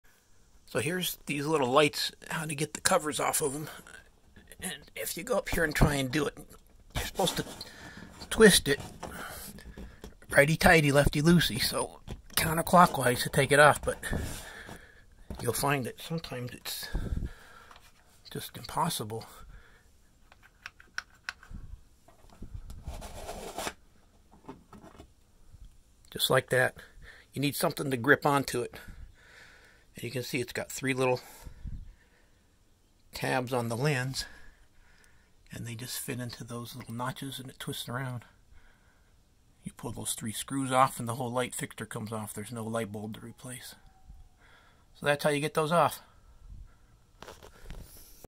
RV Dome Light Cover Removal
How to remove the cover off of the dome light in your RV trailer.